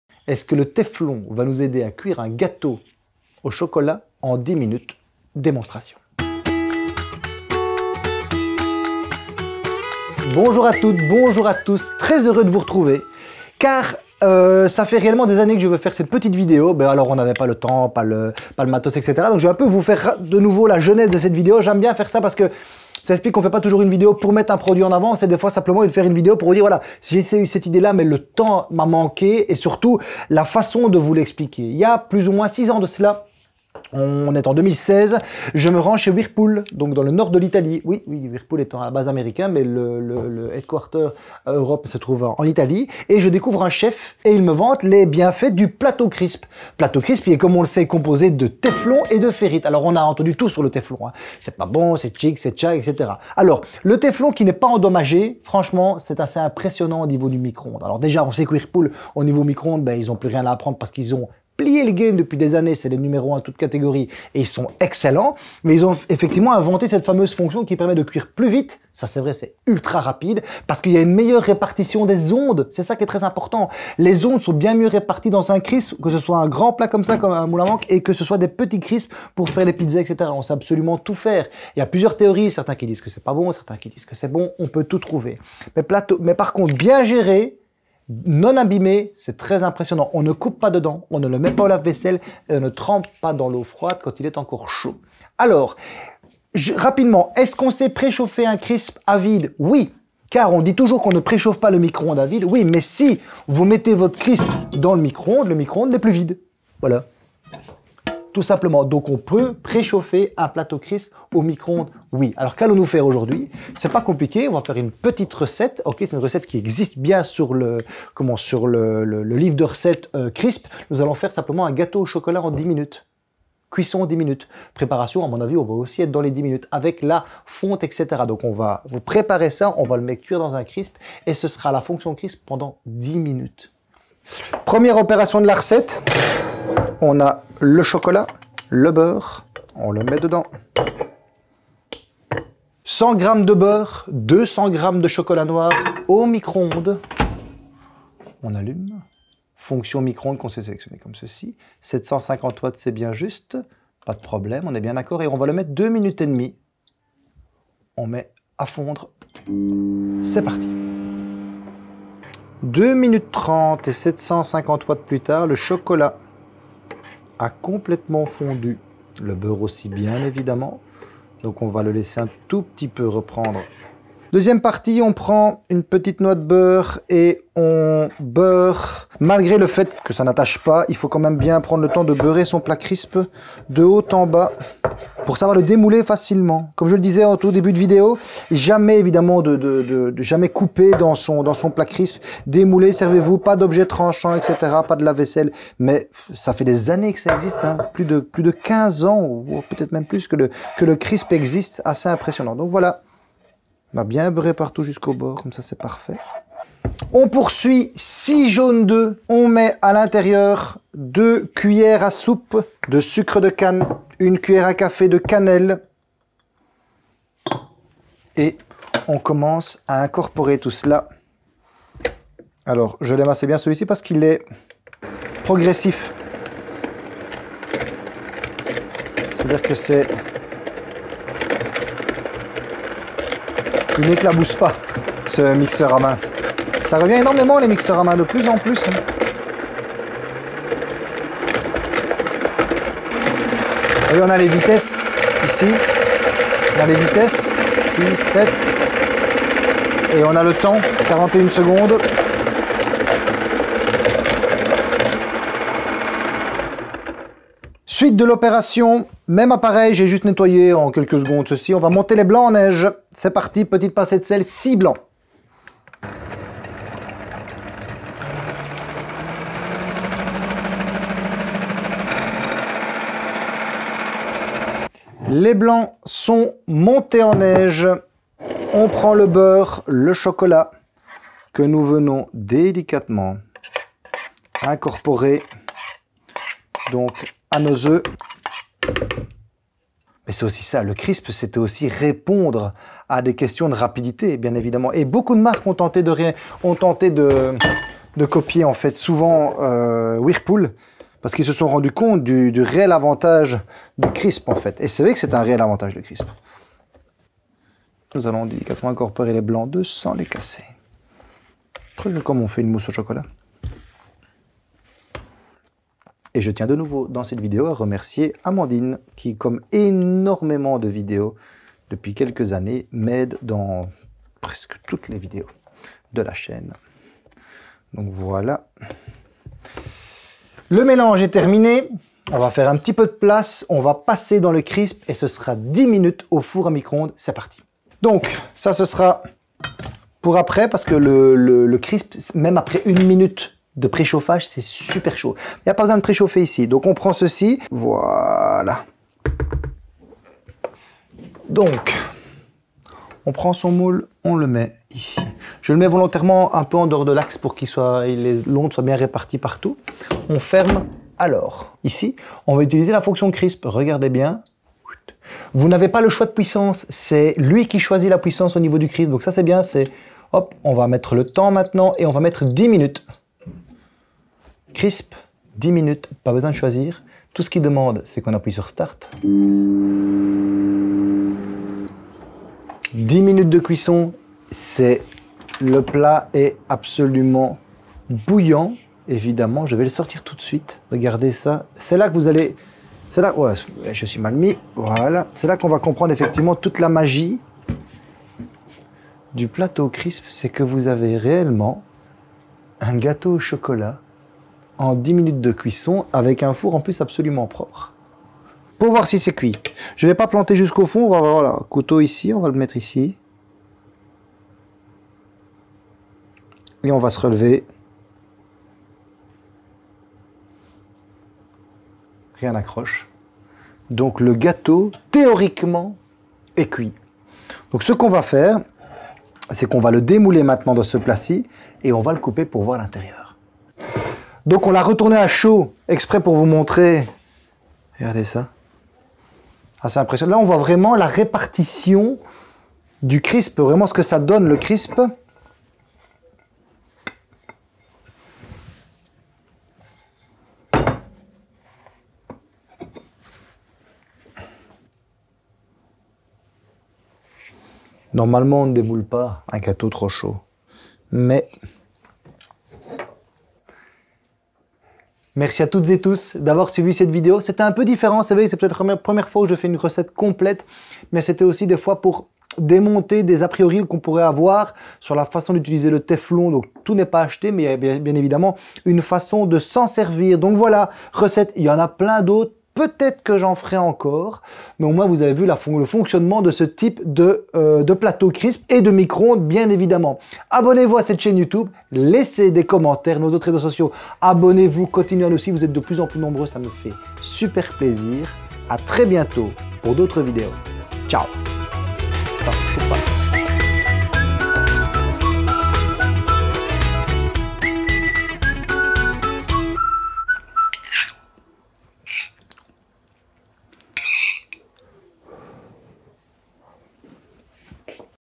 Un gâteau au micro-ondes en moins de 10 minutes !? - Micro-ondes Whirlpool avec Fonction Crisp
Oui, vous ne rêvez pas, faire un délicieux gâteau facile et rapide au micro-ondes est désormais possible avec le nouveau micro-ondes Whirlpool ! Les avantages et notre avis complet sur ce micro-ondes avec des plateaux Crisp, c'est tout de suite !

Possédant un plateau Crisp fait de téflon, cette vidéo est aussi l'occasion de démystifier la question de savoir si le téflon est dangereux pour la santé ou non.
Mais lorsqu'on se rend compte qu'avec un peu de précaution la question ne se pose pas vraiment, vous pouvez alors vous atteler à la préparation de gâteaux facile et rapide avec ce micro-onde Whirlpool aux plateaux Crisp.

Retrouvez le micro-ondes Whirlpool MWF426SL sans plateau tournant sur notre

Retrouvez le Mixeur Batteur Smeg Années'50 utilisé dans la vidéo sur notre

A bientôt,
Thomas